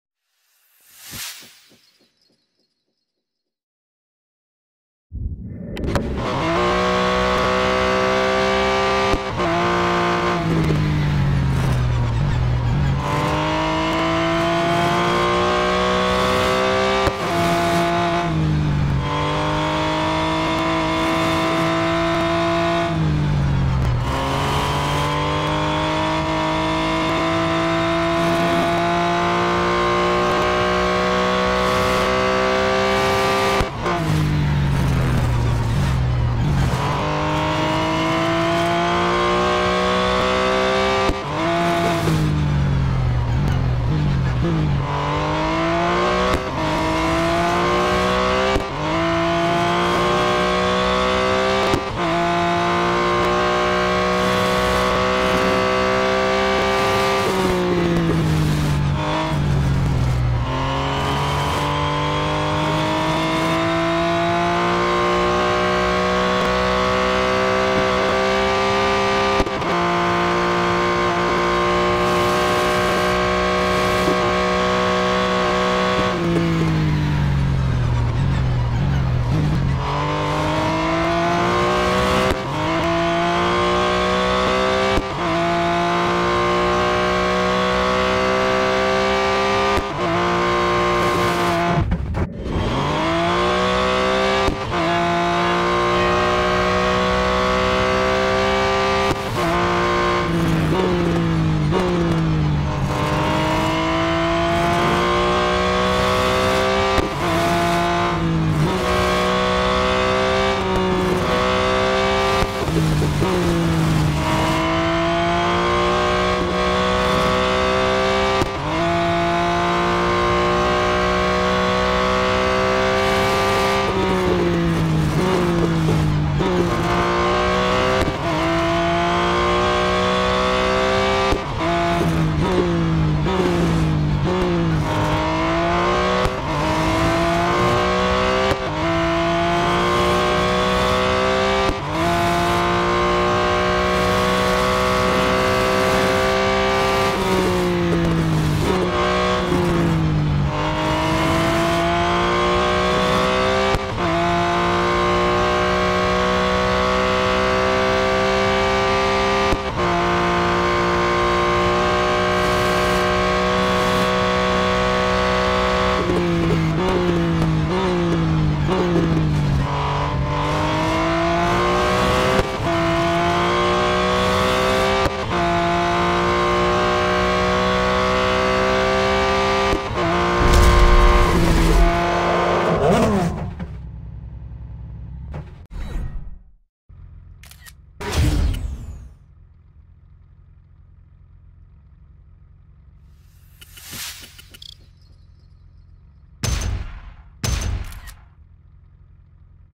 WTT (Week 2, 2024) - Porsche 911 RSR (2018) at Suzuka GP
Pretty much the maximum I can do with harsh servicing on this combo, limited drives and servicing. Looks like I’m top 26/28 upgrades RSR. There’s a couple -1 engines ahead of me. Happy with the lap, and as the hours roll by, the chances it sticks will continue to increase. Currently 20% in with 36 hours to go as of Sunday morning.
After 99 Group A’s, this is the final one in the doubles digits. Next week is going to require a miracle as the Valhalla comes at the worst time financially. Lots of farming incoming, this is not going to be fun. But it’ll be for #100; I cannot give up now.

Time: 1:24.297
Upgrades: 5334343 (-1 Engine, Drivetrain)
Ranking: 20% into Group A (-0.1 to B)